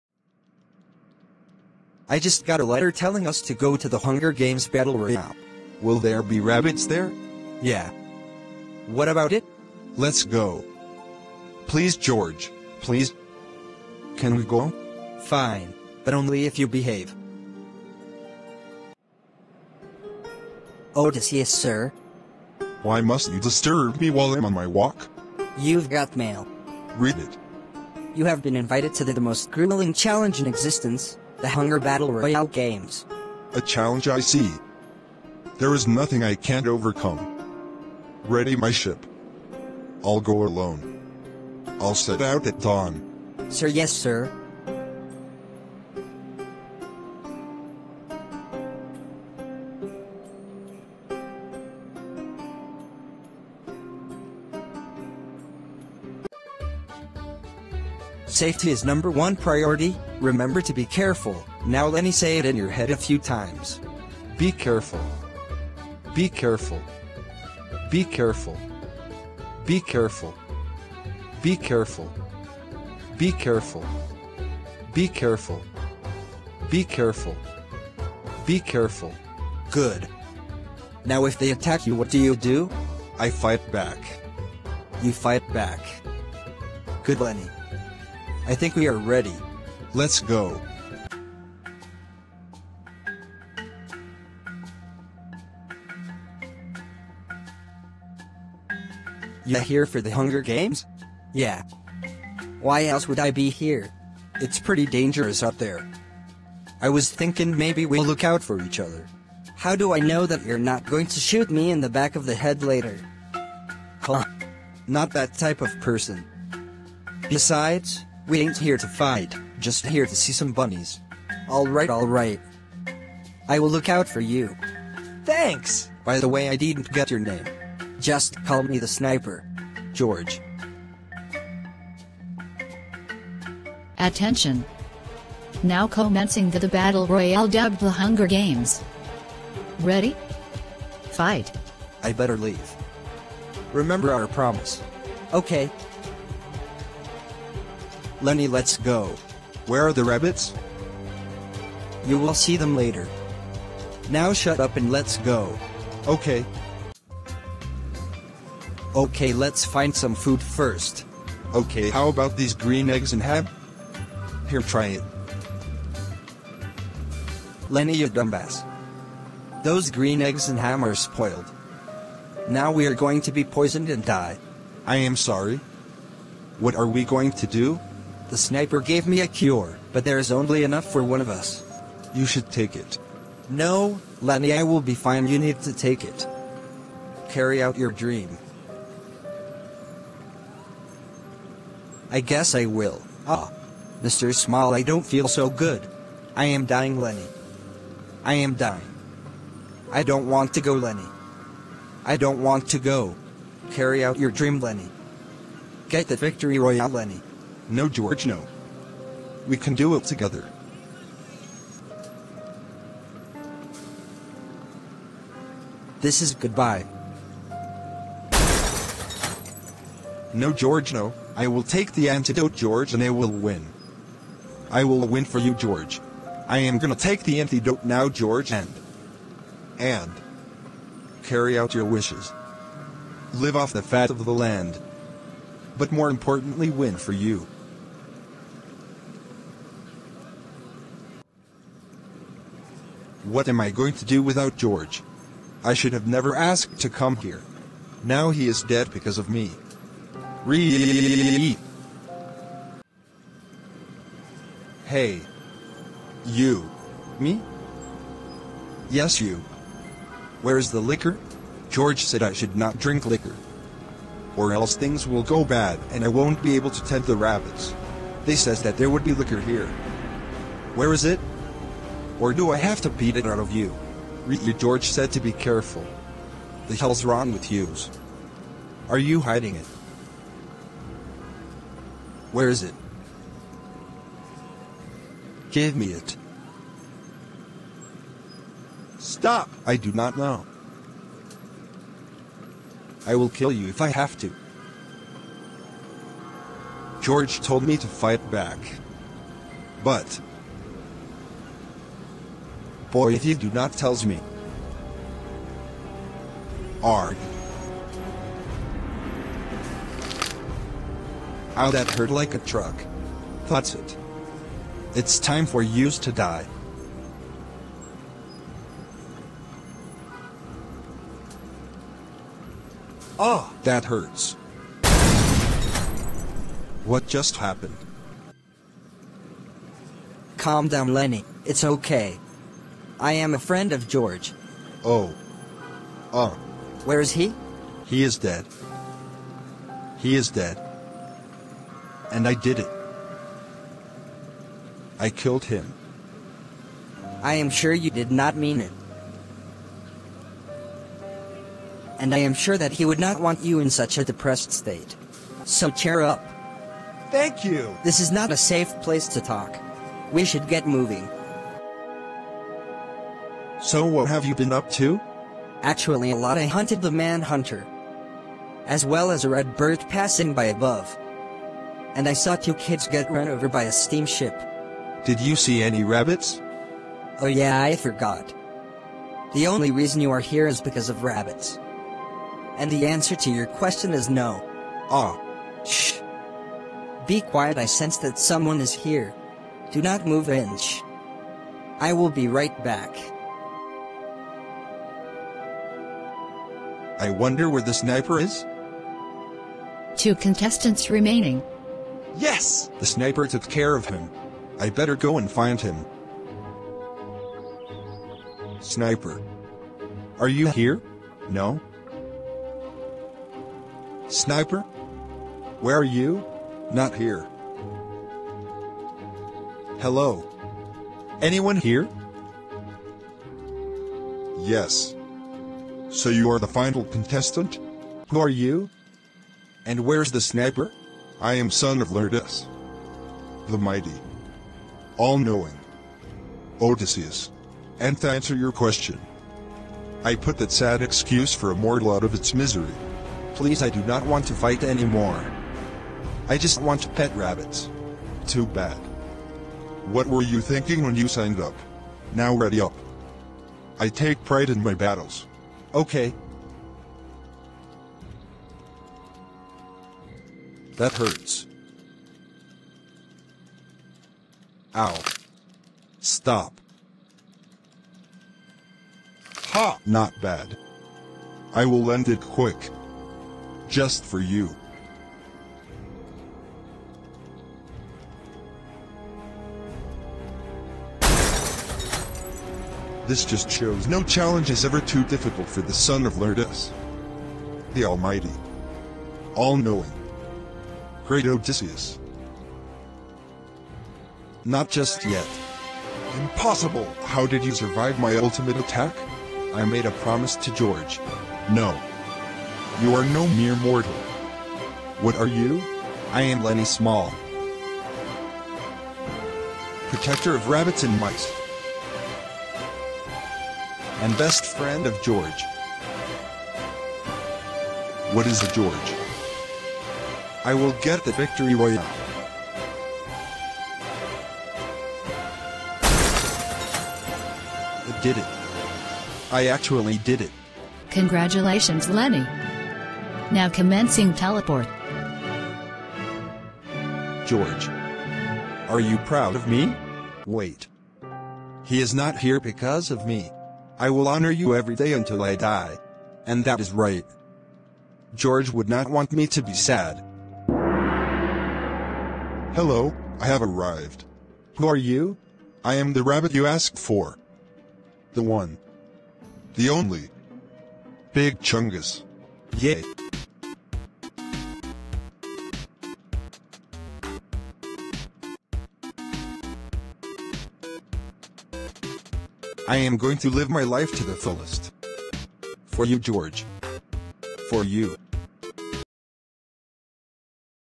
The Legend Of Lennie
This is a project for honors English 9, about the protagonists, Lennie, and George who are going on an adventure inspired by the Hunger Games in order to get the victory royale.

Credits:
Meng Shi
Max Friedman
Zach Maurice